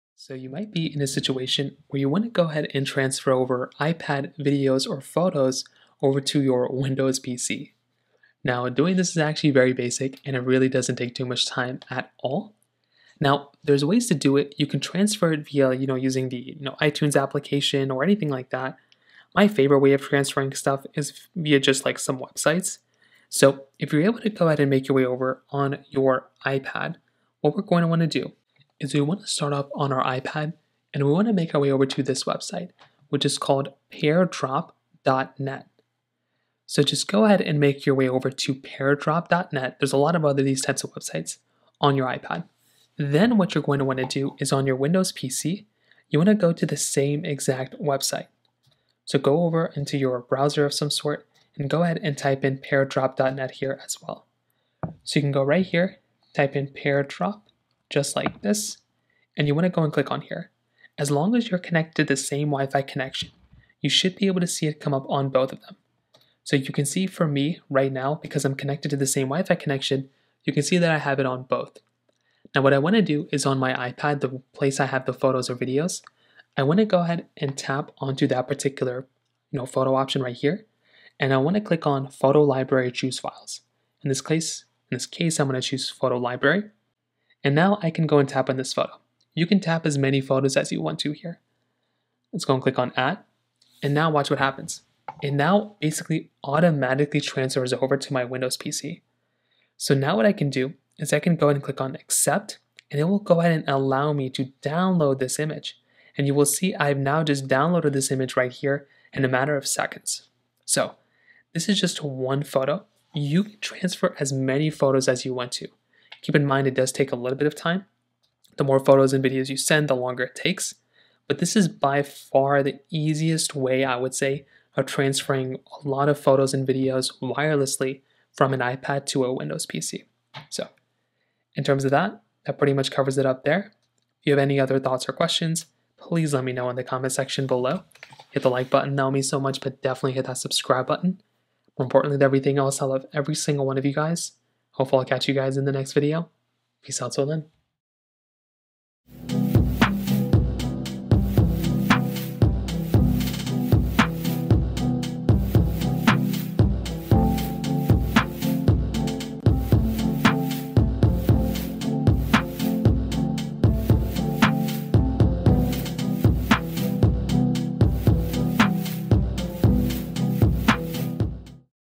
How To Transfer iPad Photos/Videos To Windows 10/Windows 11 PC! (2025)
Here is exactly How To Transfer iPad Photos/Videos To Windows 10/Windows 11 PC! (2025)